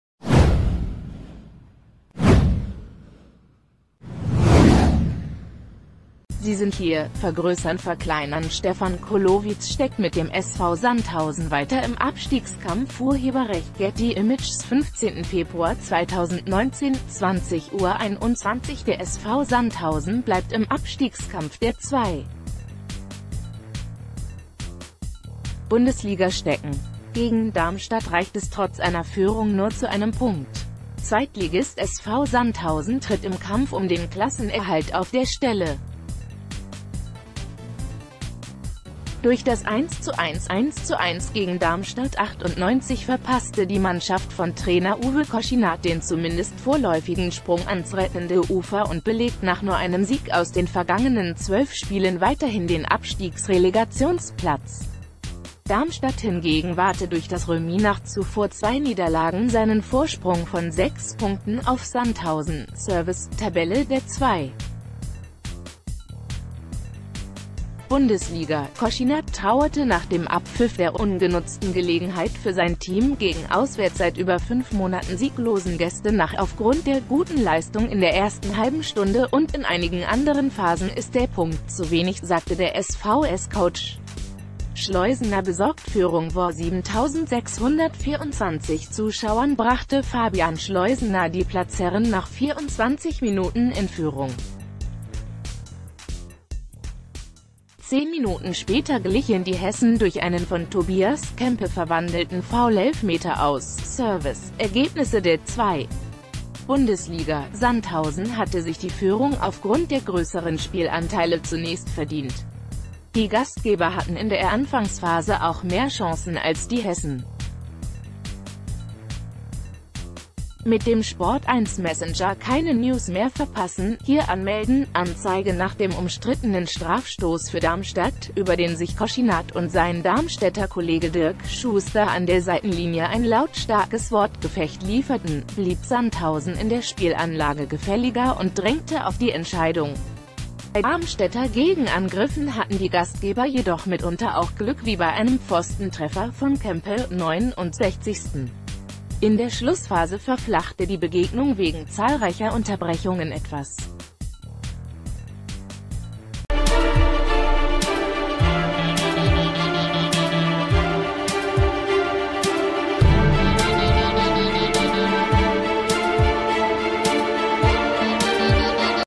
2. Bundesliga: SV Sandhausen - Darmstadt 98 1:1
2. Bundesliga: SV Sandhausen - Darmstadt 98 1:1
2. Bundesliga: SV Sandhausen - Darmstadt 98 1:1

Sie sind hier: vergrößernverkleinern Stefan Kulovits steckt mit dem SV Sandhausen weiter im Abstiegskampf © Getty Images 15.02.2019 / 20:21 Uhr Der SV Sandhausen bleibt im Abstiegskampf der 2. Bundesliga stecken. Gegen Darmstadt reicht es trotz einer Führung nur zu einem Punkt. Zweitligist SV Sandhausen tritt im Kampf um den Klassenerhalt auf der Stelle. Durch das 1:1 (1:1) gegen Darmstadt 98 verpasste die Mannschaft von Trainer Uwe Koschinat den zumindest vorläufigen Sprung ans rettende Ufer un...